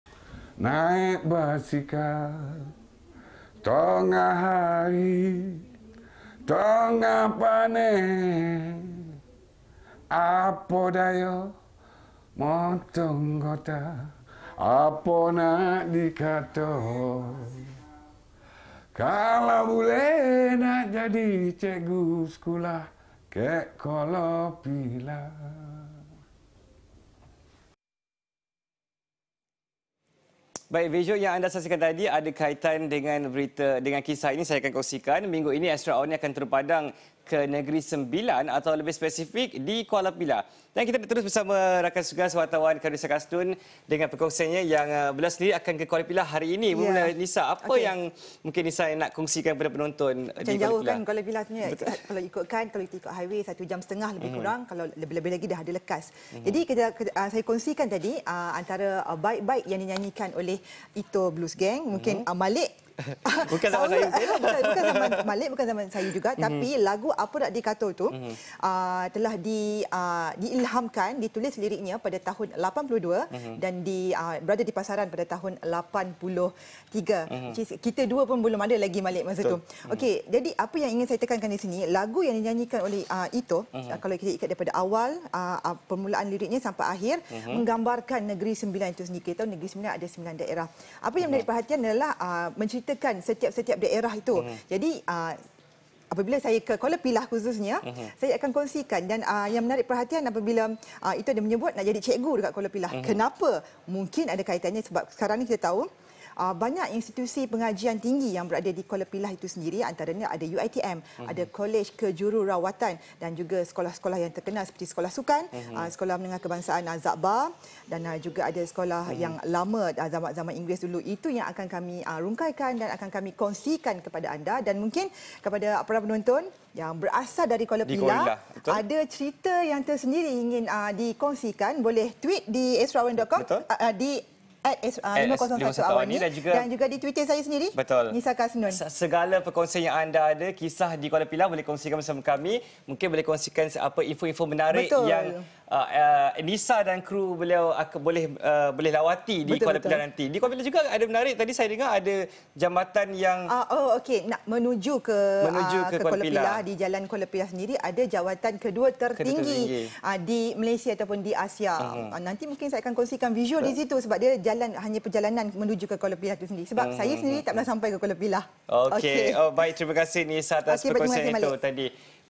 Apo ado kek Kuala Pilah?
Minggu ini Astro AWANI turun padang ke Negeri Sembilan, atau lebih spesifik di Kuala Pilah. Kita terus sahaja bersama rakan setugas, Khairunnisa Kasnoon dengan perkongsiannya.